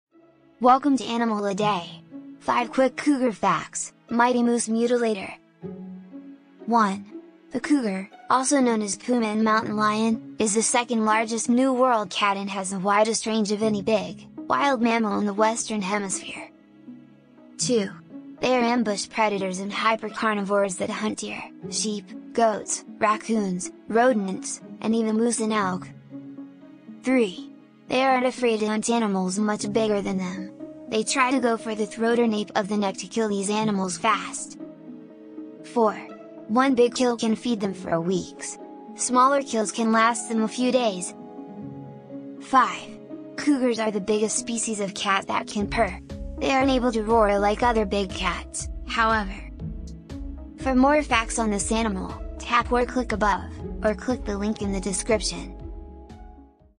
5 Quick Cougar Facts - Mighty Moose Mutilator - Animal a Day
Today we learn about the cagey cat of many names, the puma...I mean, the mountain lion...I mean, the Cougar.